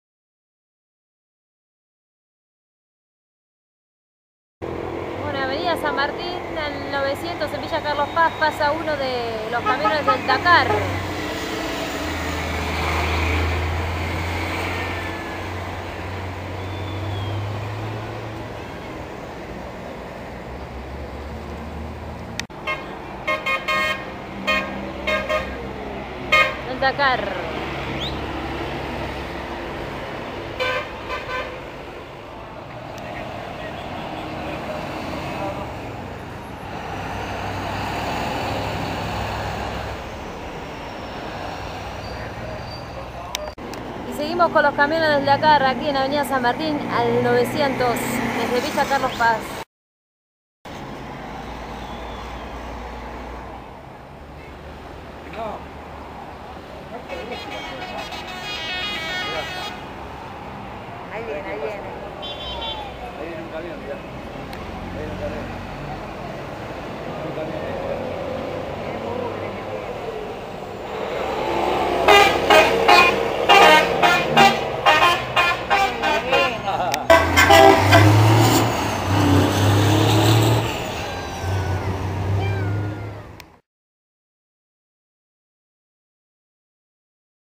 IRIDAN SAREN EN/ DAKAR CORDOBA VILLA CARLOS PAZ 2011 ESTA VEZ SIN ♥♥ OVNI ♥♥
Uno de sus pasos por la provincia de cordoba villa carlos OVNI-UFO DAKAR 2011